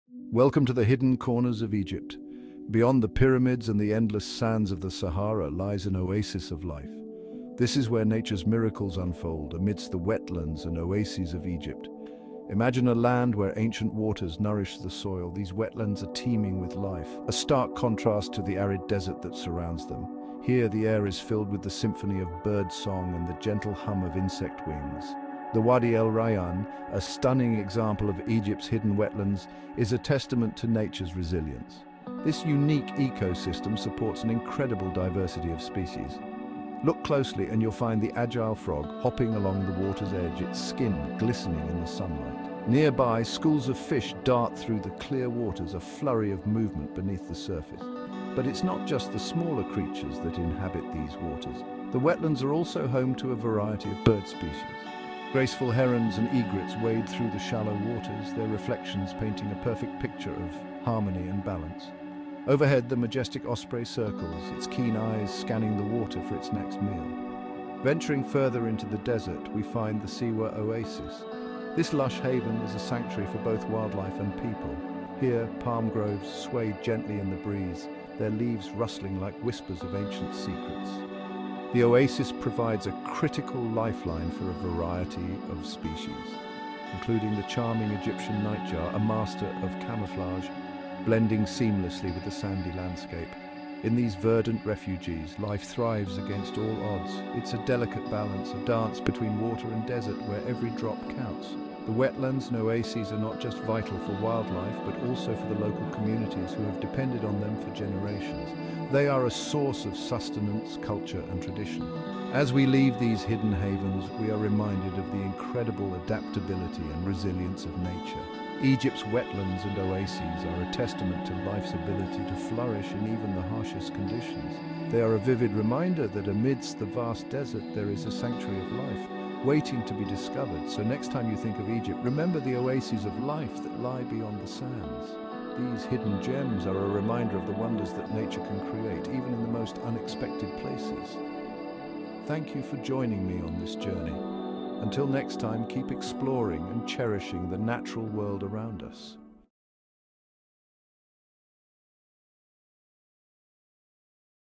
Oases of Life: Wildlife in Egypt's Wetlands and Oases
Embark on a journey to Egypt’s lush wetlands and oases, where vibrant ecosystems flourish amidst the arid desert. These verdant havens offer a stark contrast to the surrounding sands, supporting a rich tapestry of wildlife. In these wetlands, encounter colorful frogs leaping through dense vegetation and various fish darting through tranquil waters. The oases are alive with the calls of birds, such as herons and kingfishers, that thrive in these fertile niches. The unique combination of abundant water and lush plant life creates a critical refuge for these species, providing sustenance and shelter. Explore the delicate balance of these ecosystems and witness firsthand how life thrives in unexpected places. Egypt’s wetlands and oases are not only vital for the survival of these creatures but also offer a stunning example of nature’s resilience and adaptability in the face of challenging environments.